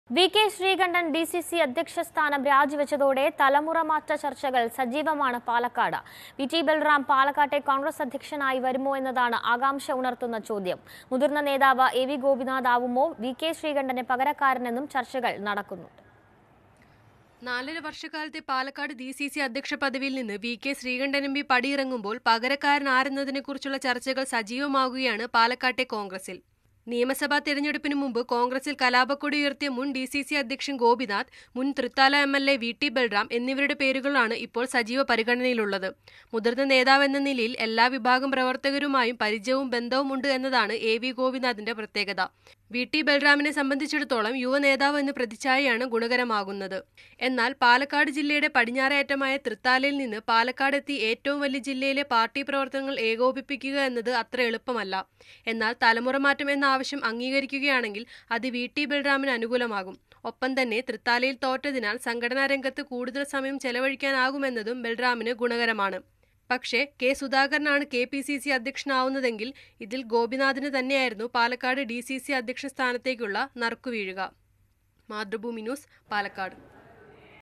വി.കെ. ശ്രീകണ്ഠന്റെ രാജി; പാലക്കാട് ഡിസിസി സ്ഥാനത്തേക്ക് അടുത്തത് ആര്? | Mathrubhumi News
വി.കെ. ശ്രീകണ്ഠന്‍ ഡിസിസി സ്ഥാനം രാജി വെച്ച പശ്ചാത്തലത്തില്‍ പാലക്കാട് ഡിസിസി സ്ഥാനത്തേക്ക് അടുത്തത് ആരെന്ന ചര്‍ച്ചകള്‍ സജീവമായി. വി.ടി. ബല്‍റാമിനും എ.വി ഗോപിനാഥിനും സാധ്യത.

Happy viewing!